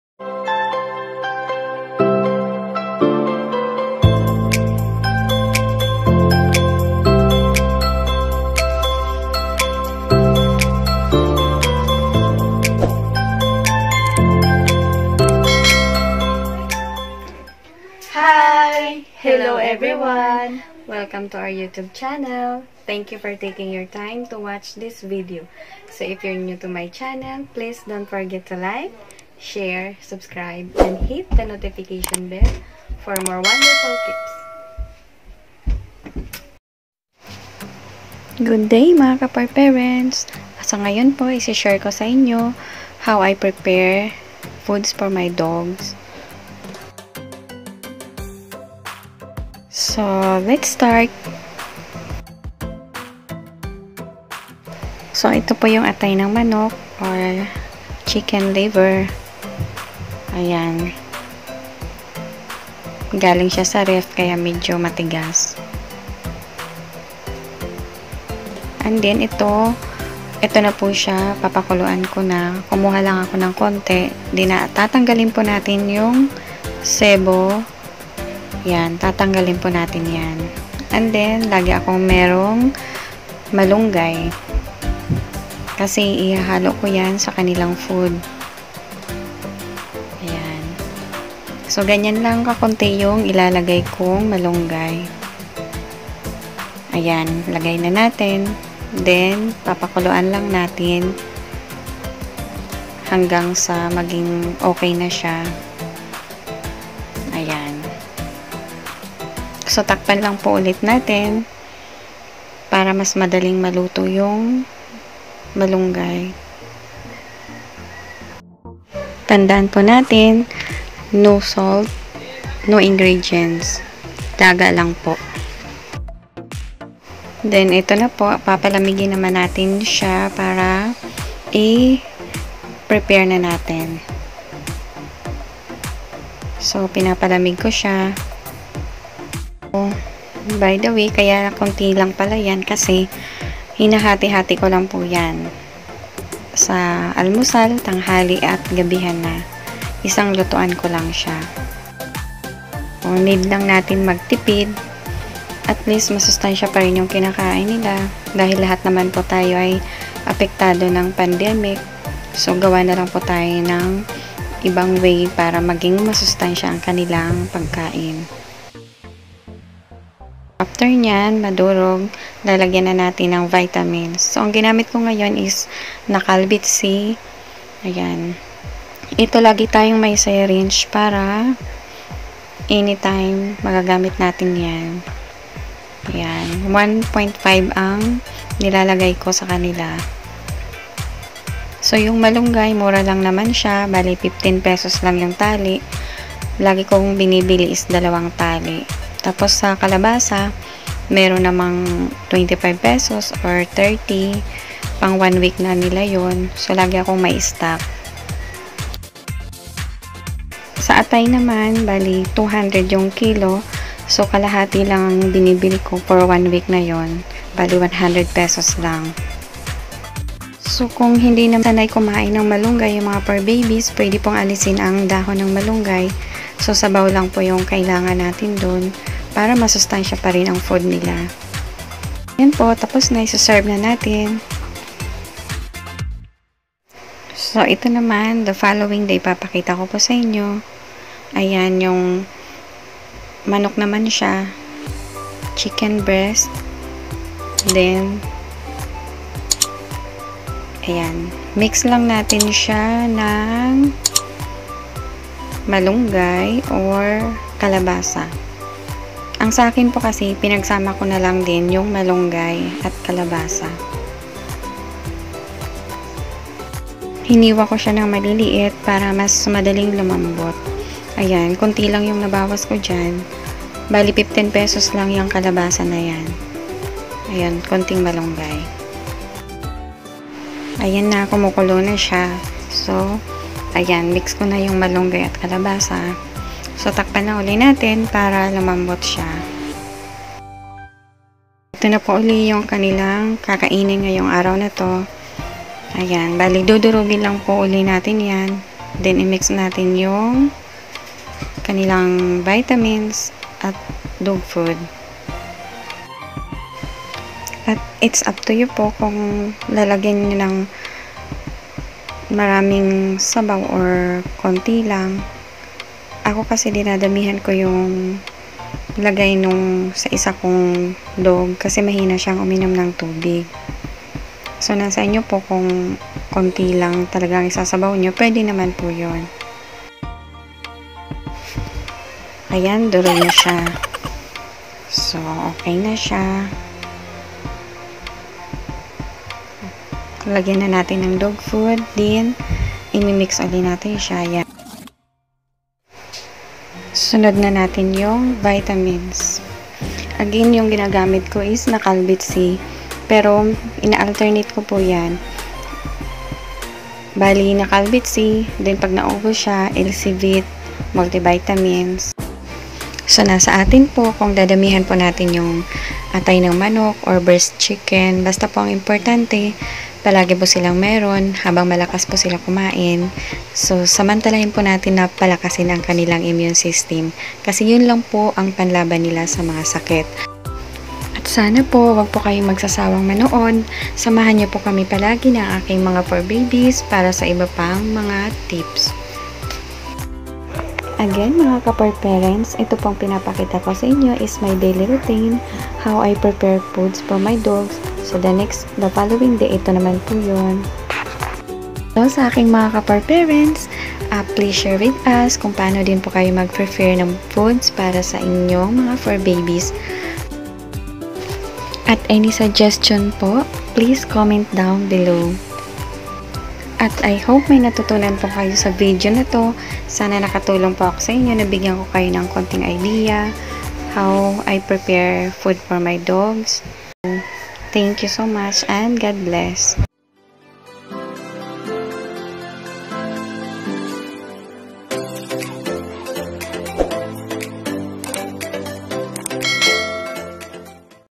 MASUSTANSIYANG PAGKAIN SA ASO O TUTA! BEST HOME MADE DOG FOOD - DAILY ROUTINE FOR MY DOGS! | SESE TV
VLOG TITLE: MASUSTANSIYANG PAGKAIN  SA ASO O TUTA! BEST HOMEMADE DOG FOOD - DAILY ROUTINE FOR MY DOG'S! | SESE TV

❇️Good Day mga PAWLALOVE'S 💗
Sa Video na Ito I share ko po..
How I prepare foods for my Dogs!
Masustansiyang Pagkain para sa mga Alaga nating ASO🐶

🐾Atay ng Manok with Kalabasa at Malunggay
🐾Breast Chicken with Kalabasa at Malunggay

Laga lang po... No Salt
No Ingredients!

Vitamins:
NaCalVit-C
LC-VIT Multivitamins Syrup

God bless 🙏